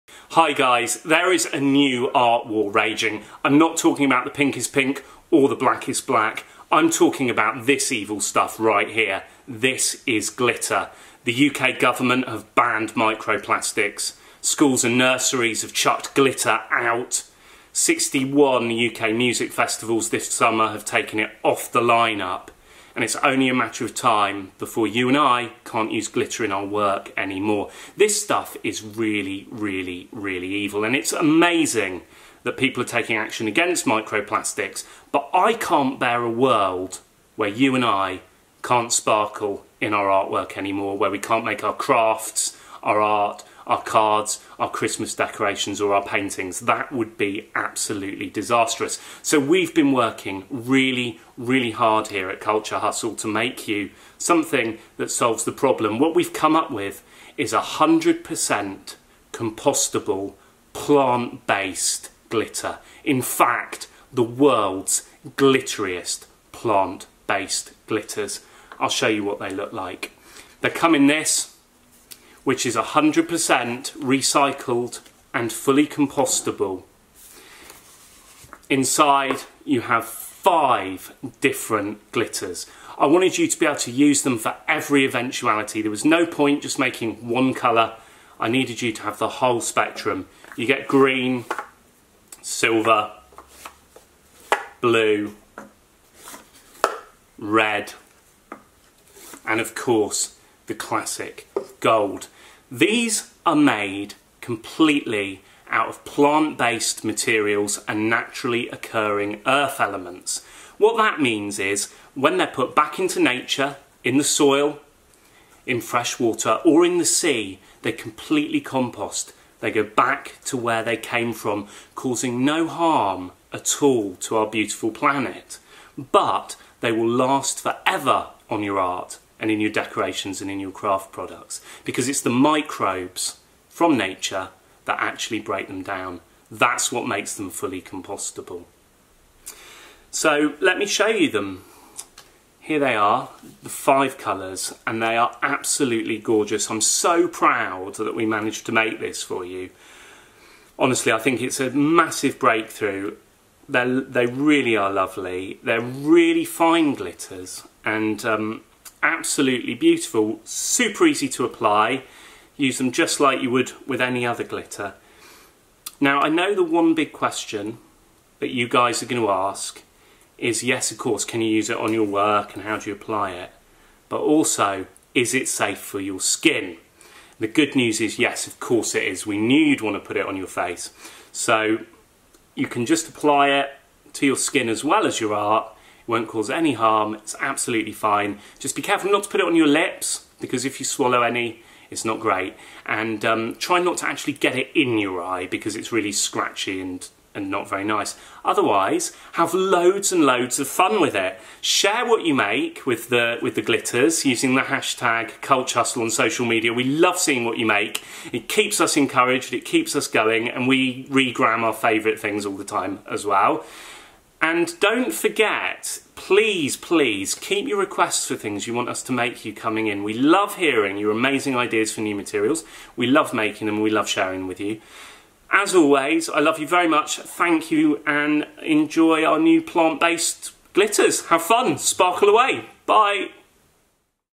Plant-based totally compostable gltteriest glitters are ready!
DAZZLE IS READY!
I’ve finished making us all a series of plant-based, compostable, super sparkly glitters.

The GLITTER WARS are real - the government have banned micro plastics, schools won’t let kids use glitters. Music festivals have said no to it too.
Glitter is so so so bad for the environment.

I didn’t want artists and makers to lose their sparkle so we’ve made these out of plants, their 100% compostable BUT last forever in your art.
We even designed totally recycled and compostable packaging to hold them!

I hope you like them and that they are useful. If we’re lucky they might save our sparkle and help the plant!

Stay sparkly! I love you!
Stuart x